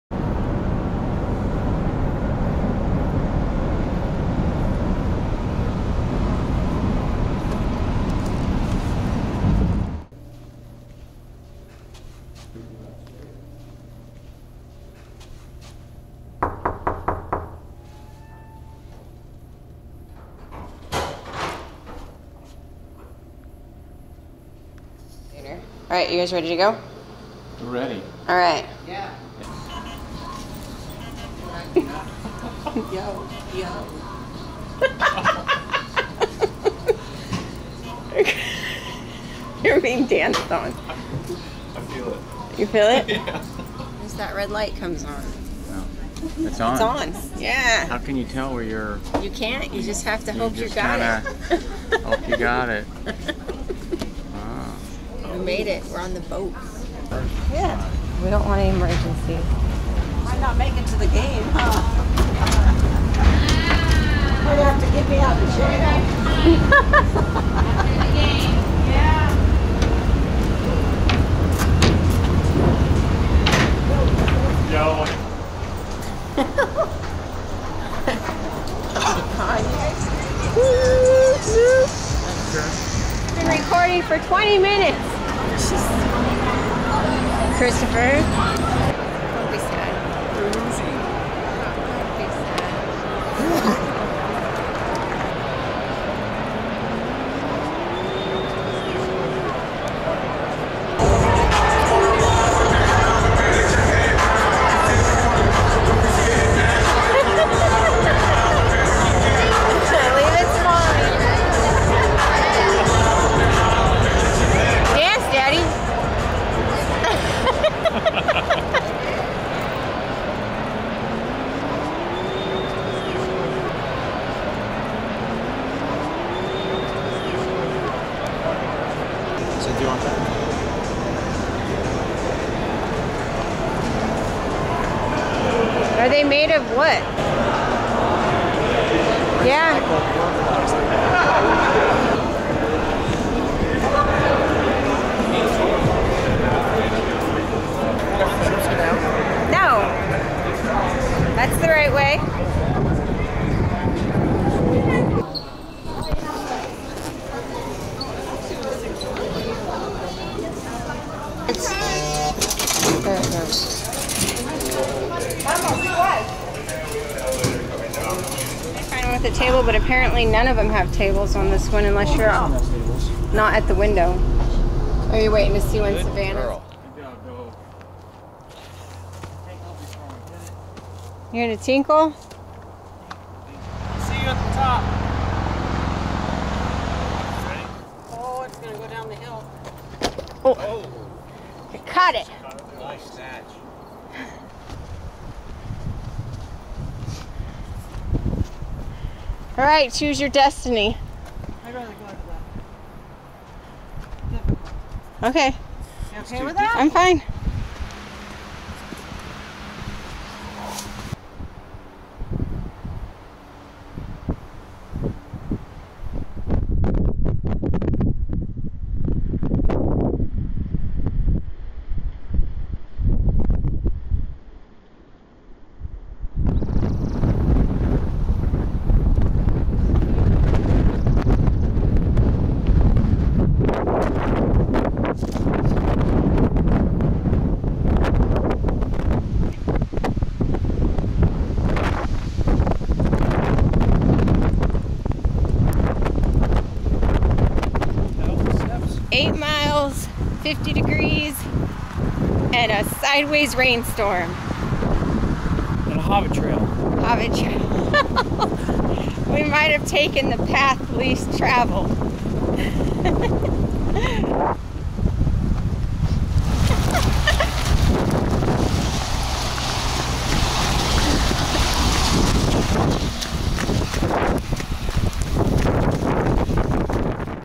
Washington 2019
We drove up to Seattle to see the Mariners vs Astros June, 2019.  On our way home we stopped at Dog Mountain to see the wildflowers.  We got caught in a storm at the top, but it was worth it!

Skip to 02:55 to see Dog Mountain hike